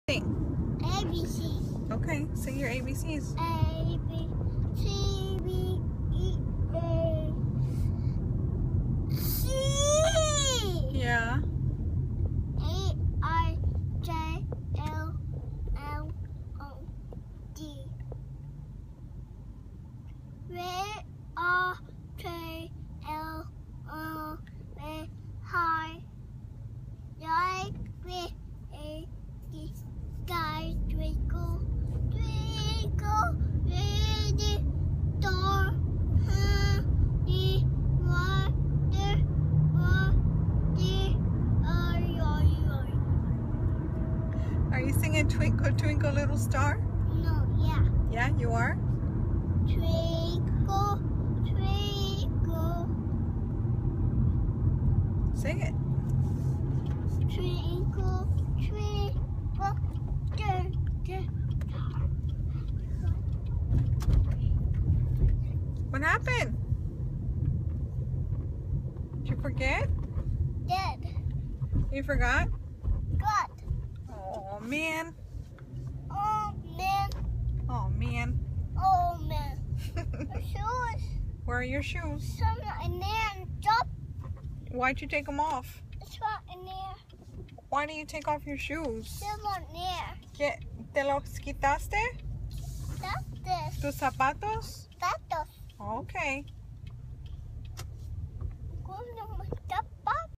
Sophia saying abc's/singing
Sophia saying her abc's and singing in the car as I take her to the babysitter. She was almost 2 years old. August 2015